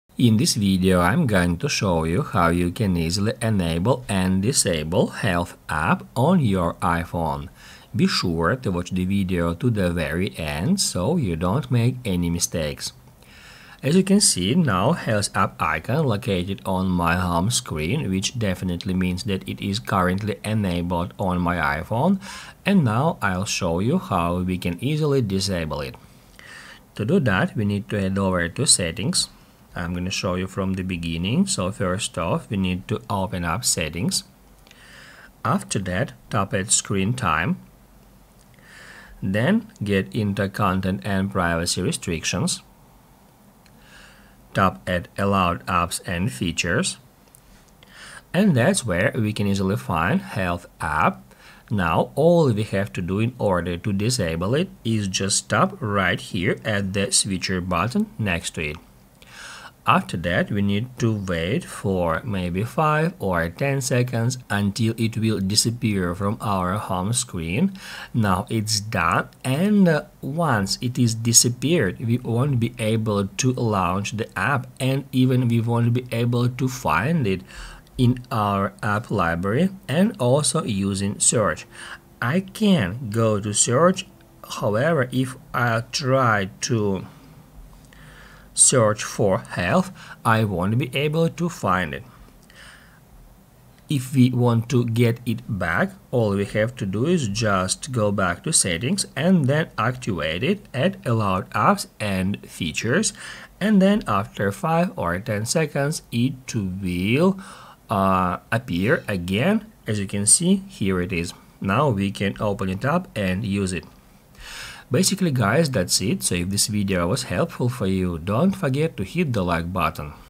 How to Enable & Disable Health App on iPhone
Here's how to enable & disable health app on iphone!